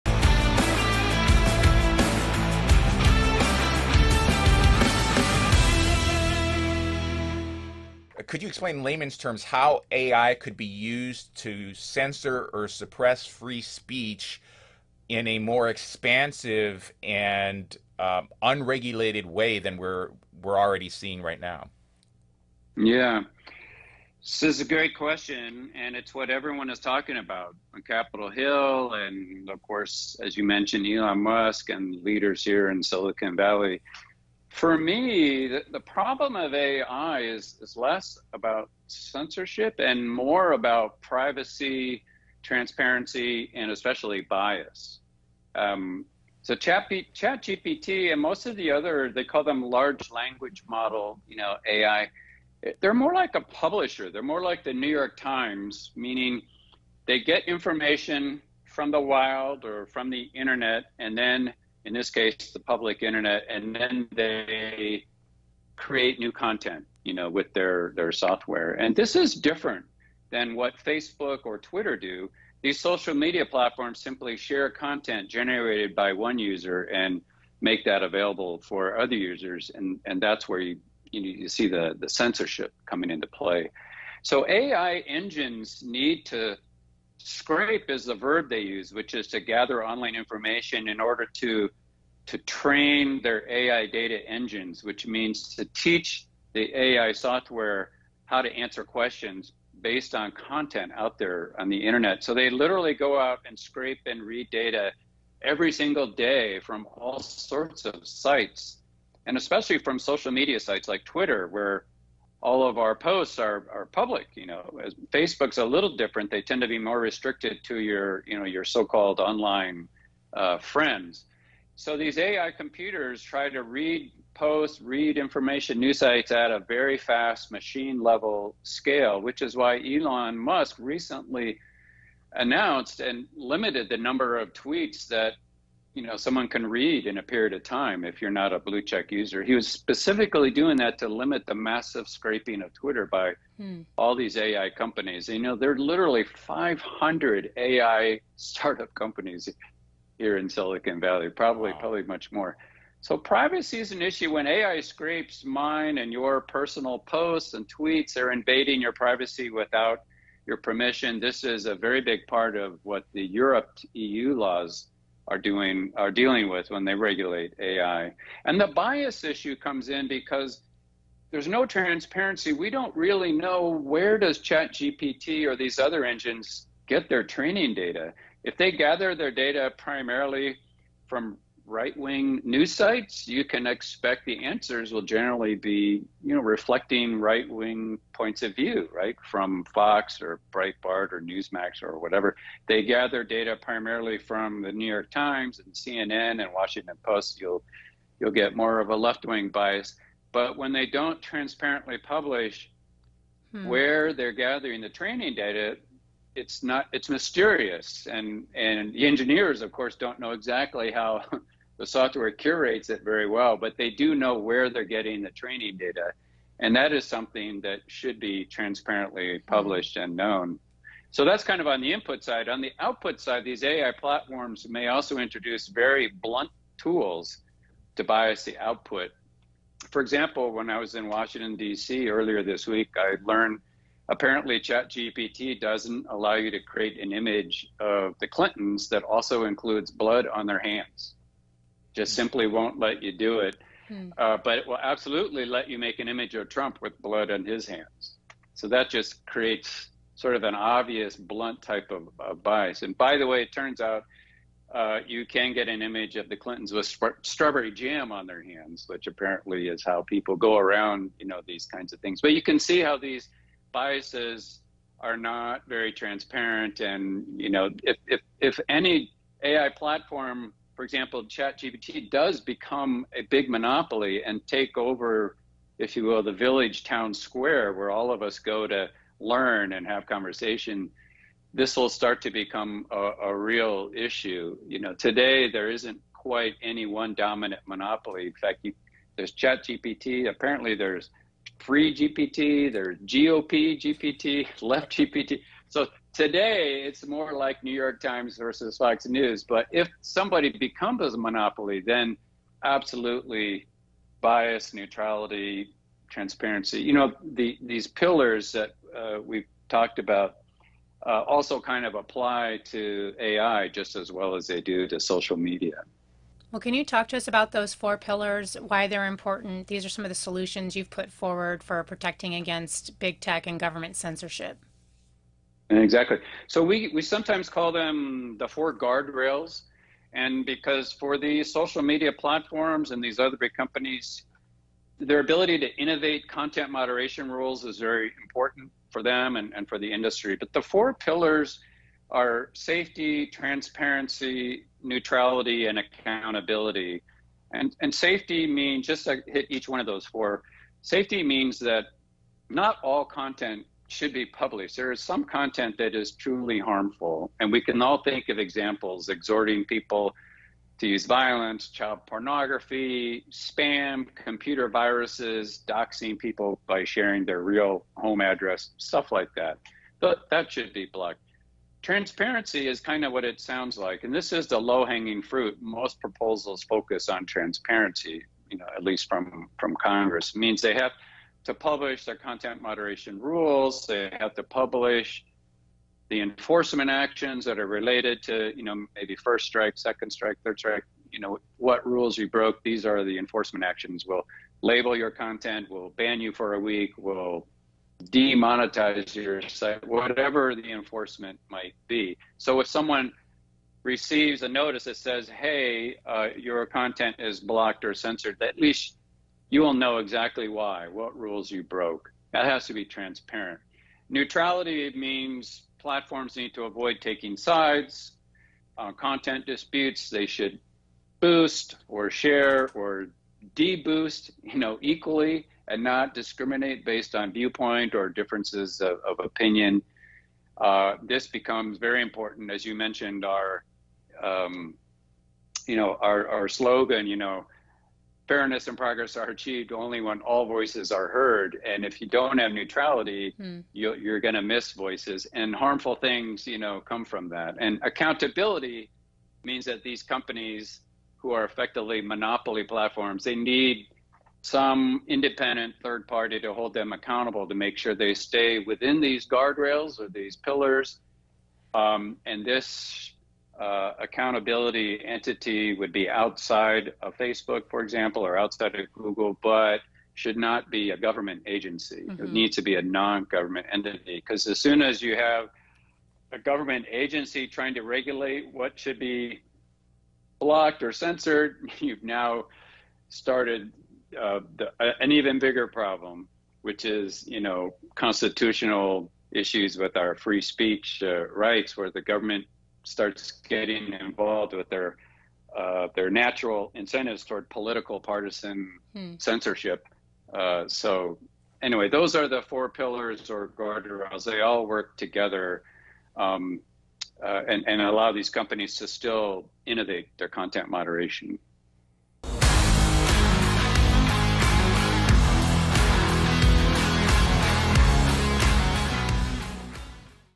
Mike Matthys on Censorship by Artificial Intelligence
As artificial intelligence advances rapidly, concerns about privacy, transparency, and bias have come to the forefront. AI engines scrape online information, including personal posts, to train their data engines, raising privacy concerns. If a single AI platform becomes a monopoly, these issues will undoubtedly become more significant.

1:35 - AI's True Threat: Privacy, Transparency, and Bias
2:54 - The AI Arms Race: Scraping the Internet for Data
4:54 - Seeing the Invisible Hand: AI's Blunt Bias Controls
6:38 - The Four Pillars: Guardrails for Responsible Content Moderation
8:06 - The Neutrality Imperative: Avoiding Viewpoint Discrimination